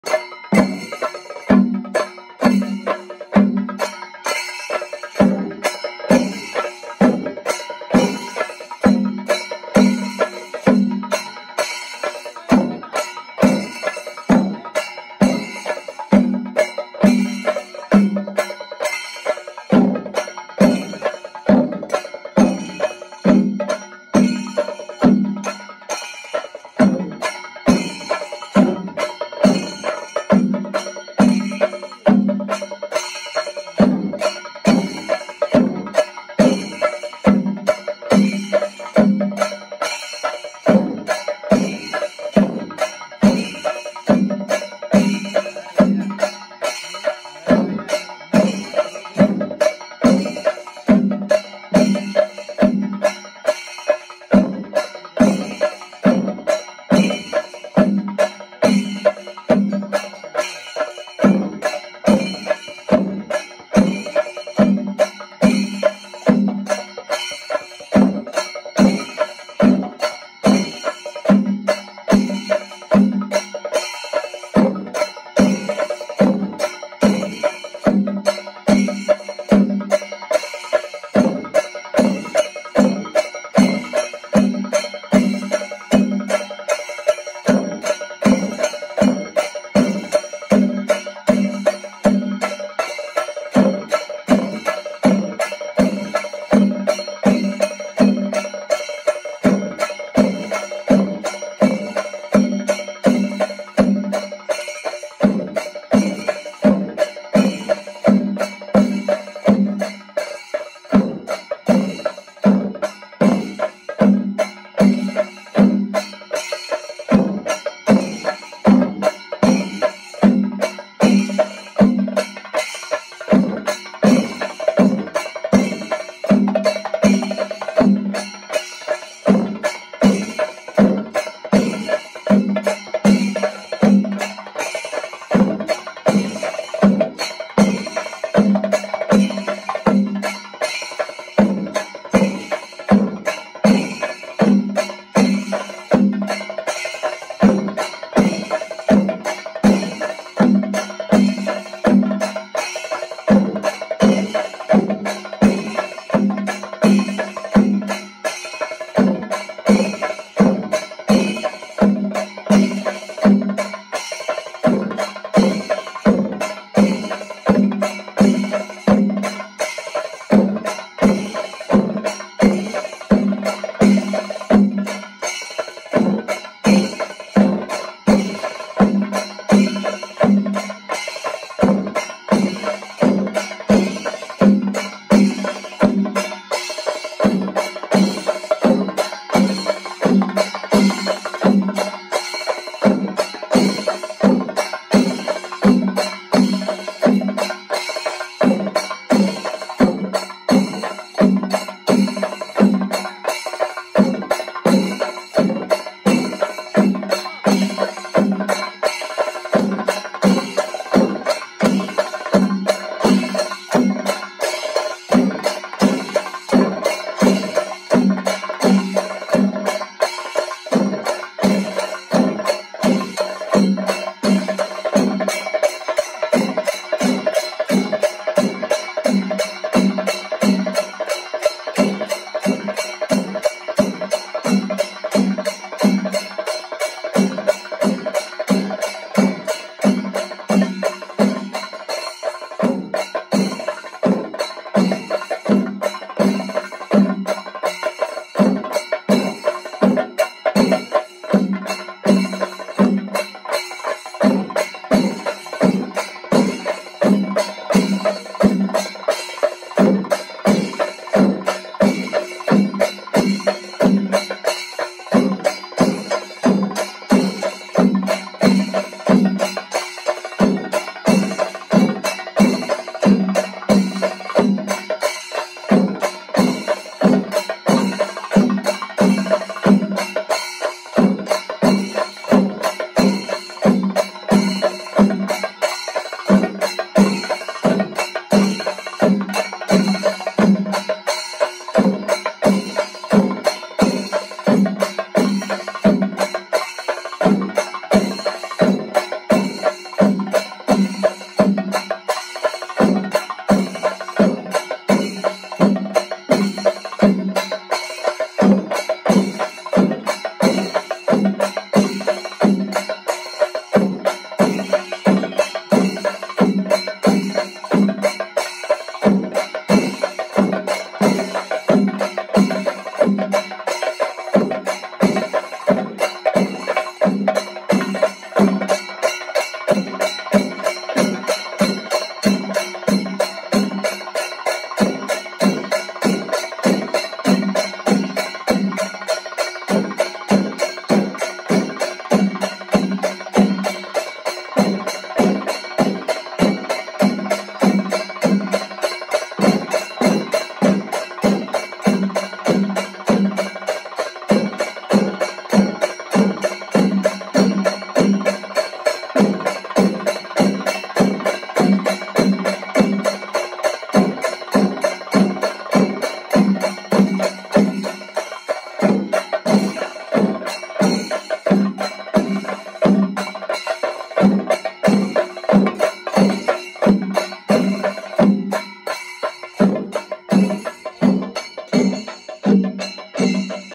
Koodalmanikyam Utsavam 2022 - Valiya Vilakk - Chembada - Vaka Vayikkal - Peruvanam Kuttan Marar
Chembada Vaka Vayikkal

Kula pradikshanam

Irinjalakuda Rajeev Warrier

Chowallur Mohanan
Peruvanam Anil

Peruvanam Kuttan Marar
Peruvanam Karthik

Melam lead by Peruvanam Kuttan Marar

Valiya Vilakk Sheeveli

Koodalmanikyam Utsavam 2022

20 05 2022

Thidambu Paramekkavu Kashinathan